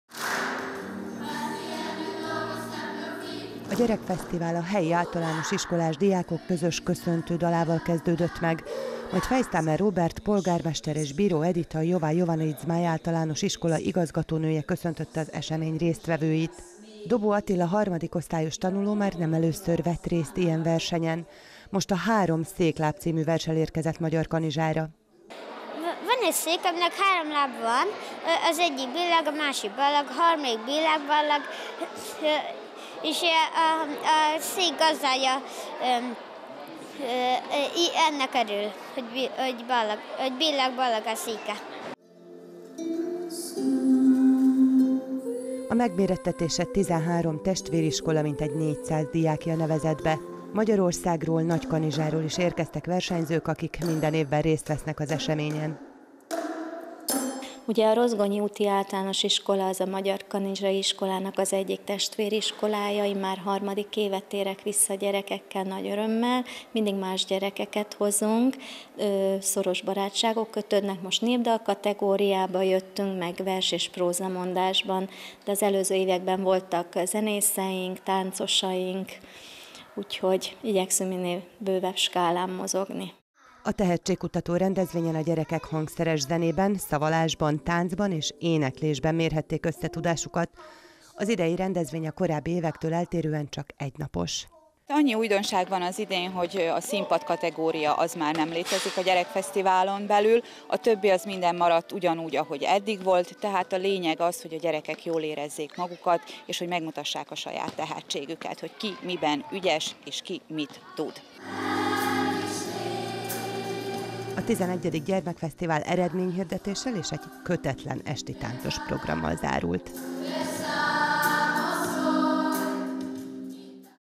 2018.04.20. - Gyermekfesztivál Magyarkanizsán
Mintegy 400 diák vett részt a 11. Gyermekfesztiválon, amelyet a Jovan Jovanović Zmaj Általános Iskola tornatermében tartottak. A gyerekek idén is 14 kategóriában mérhették össze tudásukat.